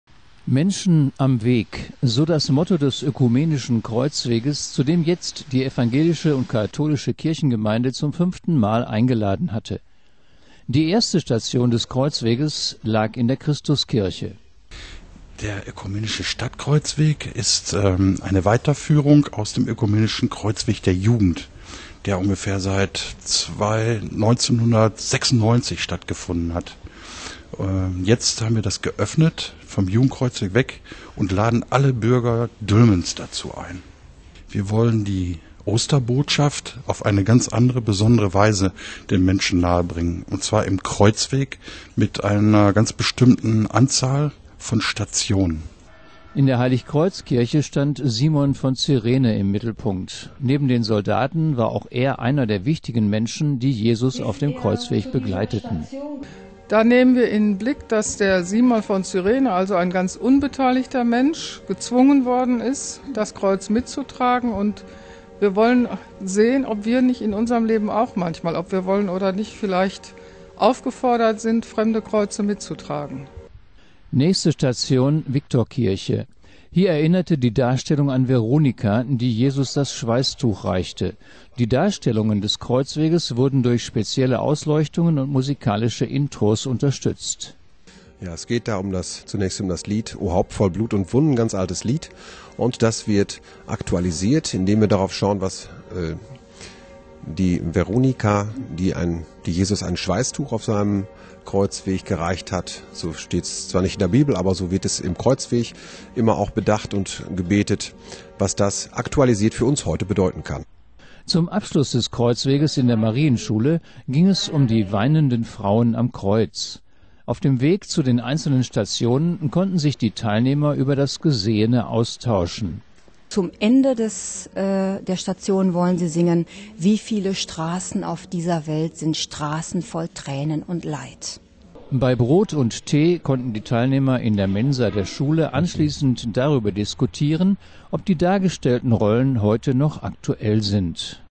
Ökomenischer Kreuzweg in Dülmen
Vier Stationen beinhaltete der Ökomenische Kreuzweg in Dülmen. Start war an der Christuskirche in Dülmen.